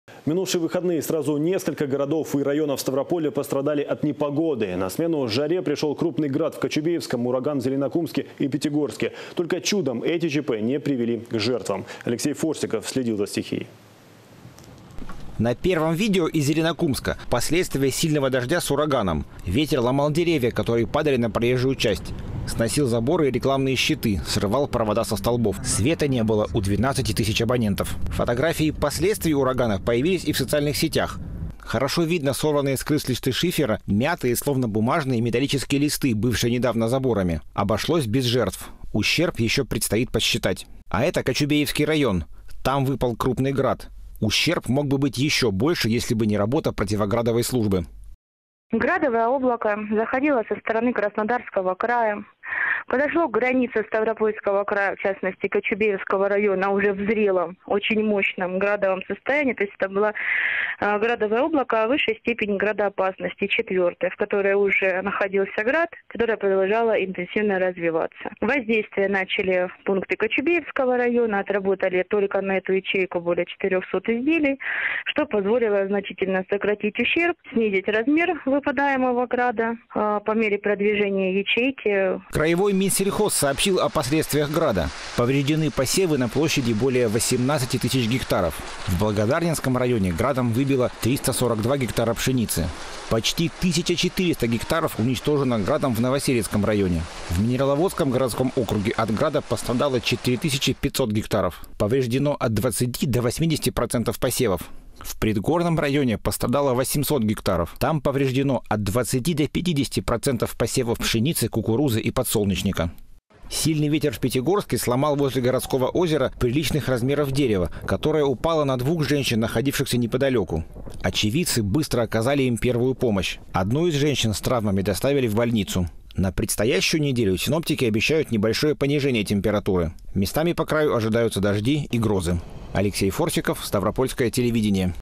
В Ставропольском крае подсчитывают убытки от удара стихии
В минувшие выходные сразу несколько городов и районов Ставрополья пострадали от непогоды. На смену жаре пришел крупный град в Кочубееском, ураган в Зеленокумске и Пятигорске. Только чудом эти чп не привели к жертвам. Алексей Форсиков следил за стихией.